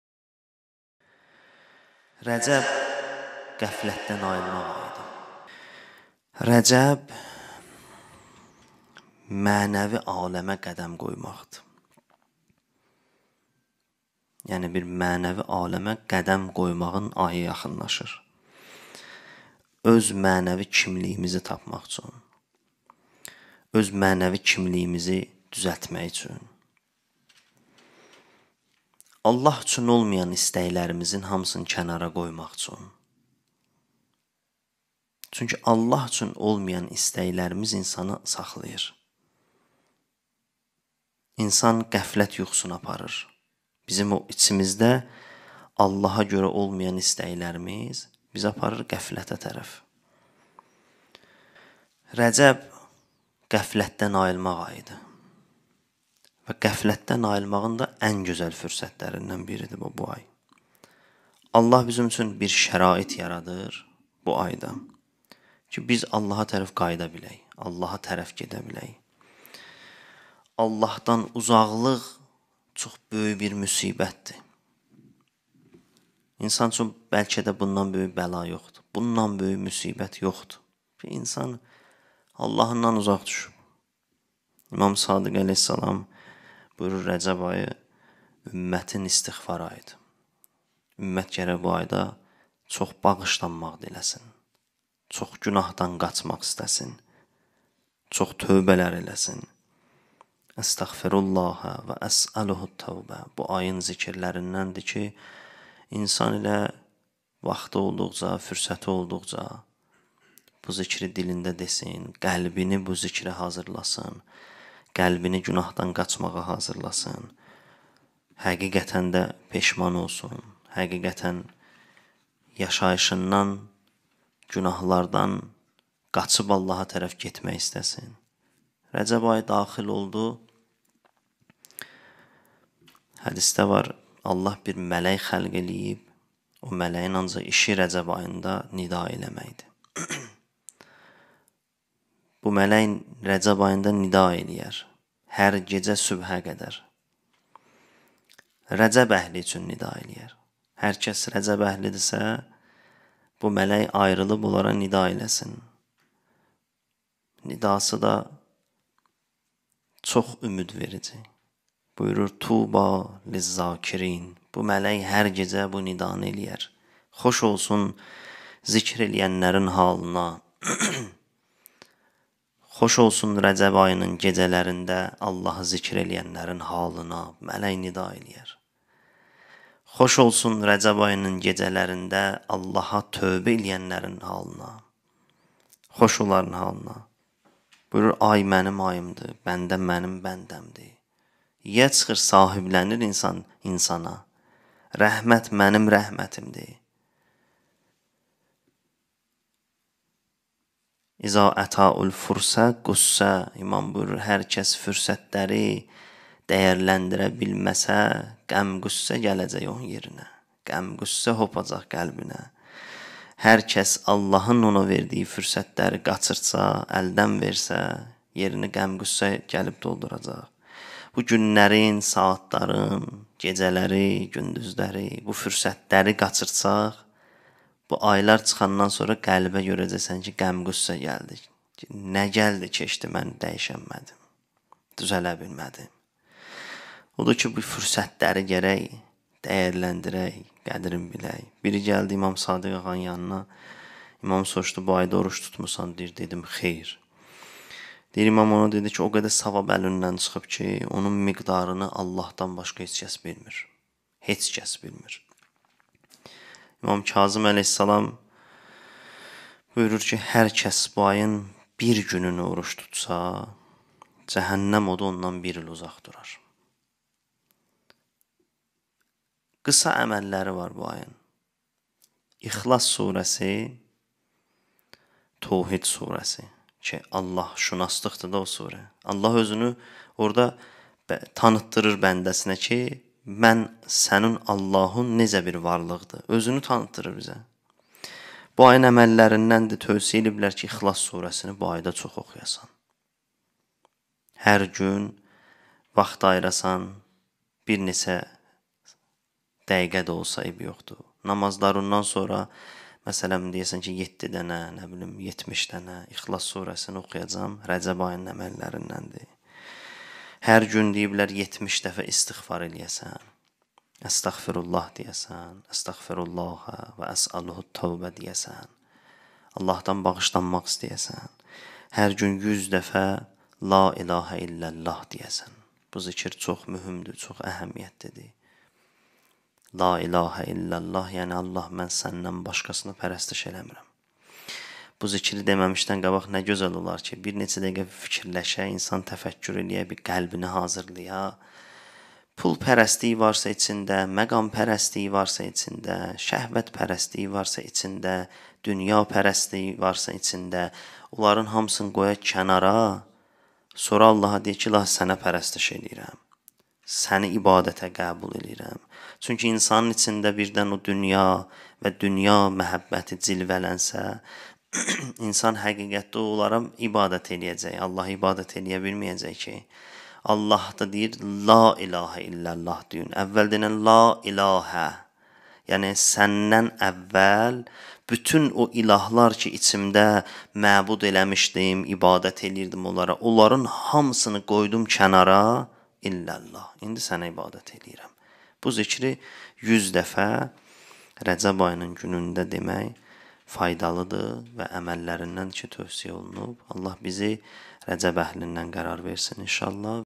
Hacı Surxay - Rəcəb ayının fəziləti və əməlləri
Hacı Surxay Əhmədov bu xütbədə Rəcəb ayının əməlləri mövzusunda bi sıra məsələlərə toxundu.